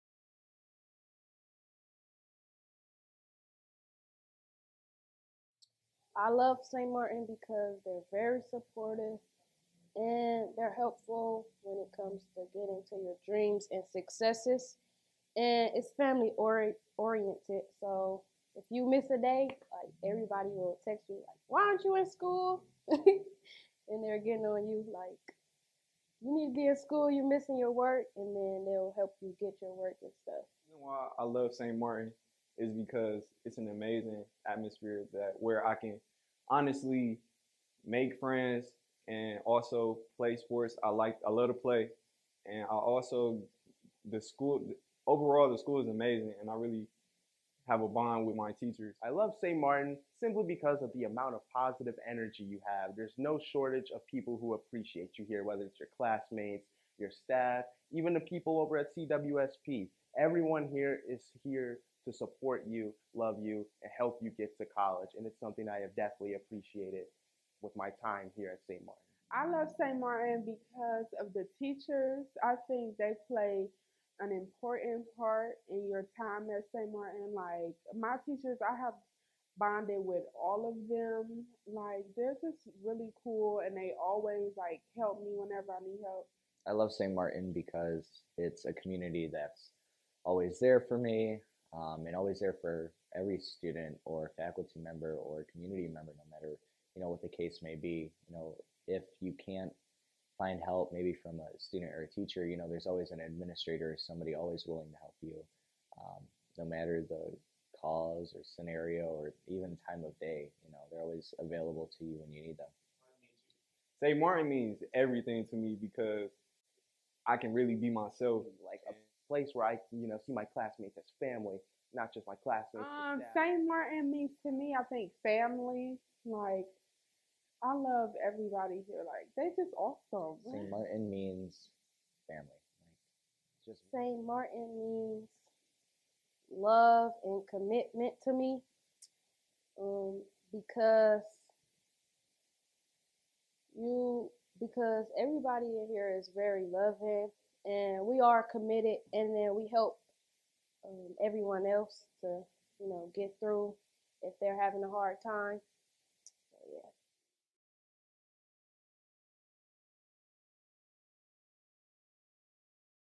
Why I Love Saint Martin...
Students share about why they love Saint Martin and what it means for them to be part of the school community.